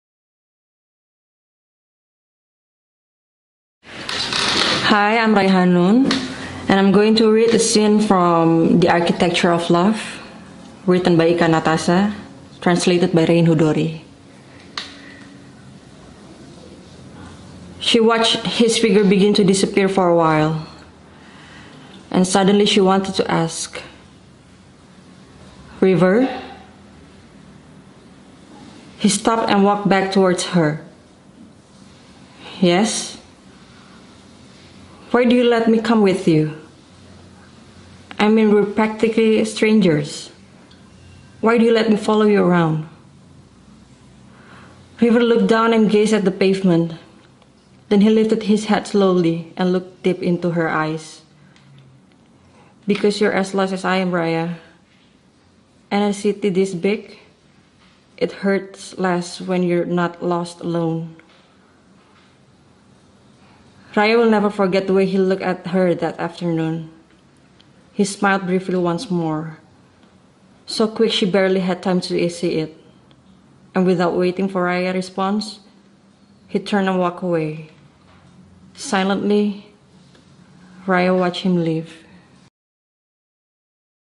Raihaanun reading an excerpt from The Architecture of Love
”Because you’re as lost as I am, Raia. And in a city this big, it hurts less when you’re not lost alone.”

Raihaanun reading a scene from TheArchitectureofLove, now in bookstores.